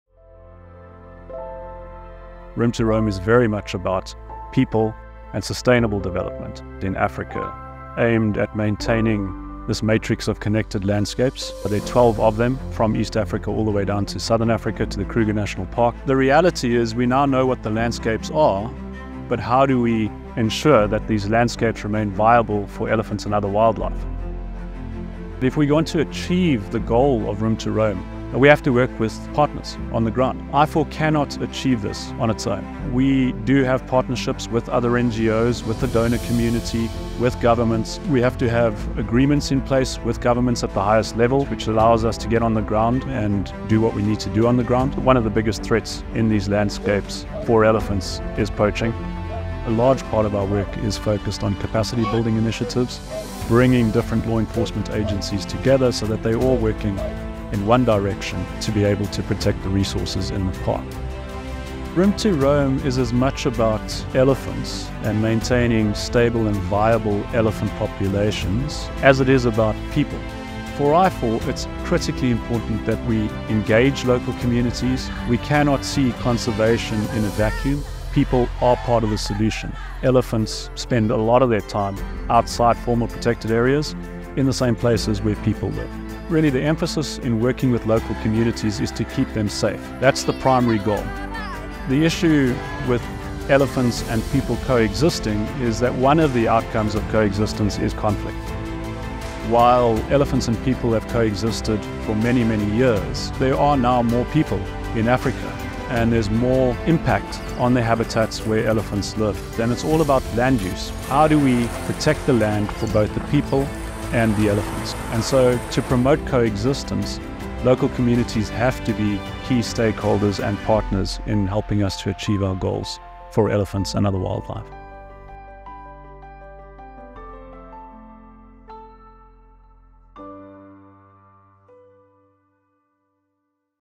Room to Roam: enabling people and wildlife to co-exist
Climate change poses a looming threat to wild animals and people in Africa. Through community engagement with locals and traditional leaders, IFAW works to help animals, people, and the planet. People living alongside wildlife often become involved with human-wildlife conflict, a major issue threatening many endangered species. But when people adopt wildlife-friendly livelihoods and new agricultural techniques, people and animals can coexist the thrive together.

About IFAW:

IFAW explores new ways to improve conditions for animals, people, and the place we call home.